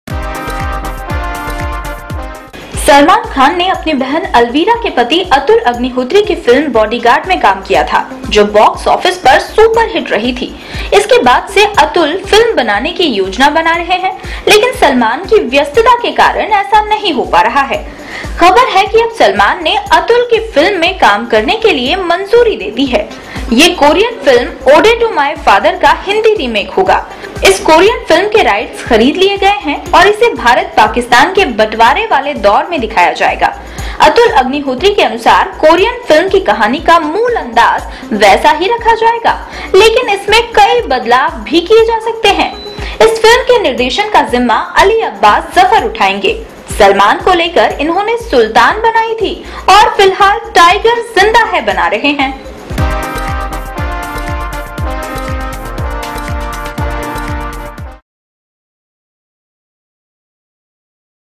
कोरियन फिल्म के रिमेक में सलमान खान करेंगे काम
यह कोरियन फिल्म 'ओडे टू माय फादर'  का हिंदी रिमेक होगा।  इस कोरियन फिल्म के राइट्स खरीद लिए गए है और इसे भारत-पाकिस्तान के बंटवारे वाले दौर में दिखाया जाएगा। अतुल अग्निहोत्री के अनुसार कोरियन फिल्म की कहानी का मूल अंदाज वैसा ही रखा जाएगा, लेकिन इसमें कई बदलाव भी किए जा सकते हैं।
 इस फिल्म के निर्देशन का जिम्मा अली अब्बास ज़फर उठाएंगे। सलमान को लेकर इन्होंने 'सुल्तान' बनाई थी और फिलहाल 'टाइगर जिंदा है' बना रहे हैं।